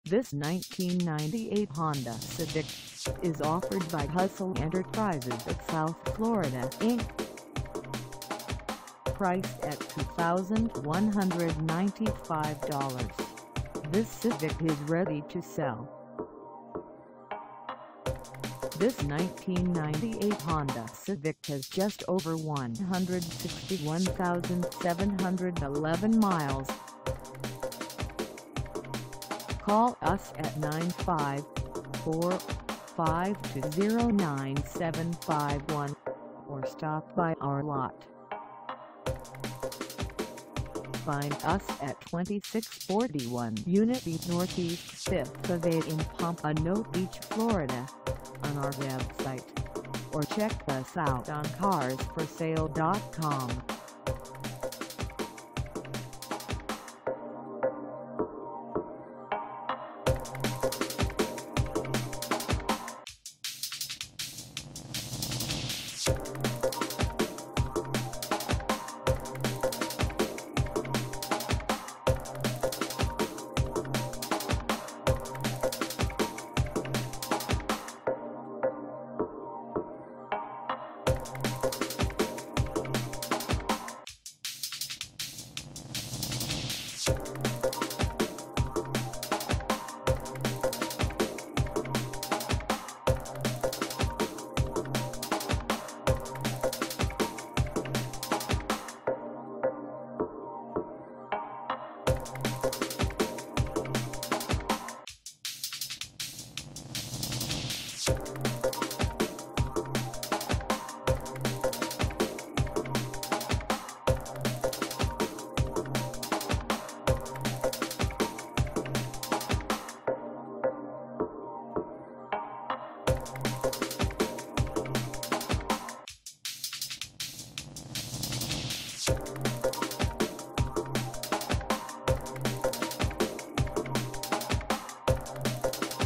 1998 Honda Civic EX 2dr Coupe for sale in Pompano Beach, FL
This 1998 Honda Civic EX 2dr Coupe is for sale in Pompano Beach, FL 33064 at Hustle Enterprises of South Florida Inc.
Engine: I4 1.6L I4
Transmission: Automatic 4-Speed
Color: GREEN
Mileage: 161711
Contact Hustle Enterprises of South Florida Inc 2641 unit B North East 5th Ave Pompano Beach, FL 33064 to test drive this 1998 Honda Civic EX 2dr Coupe today.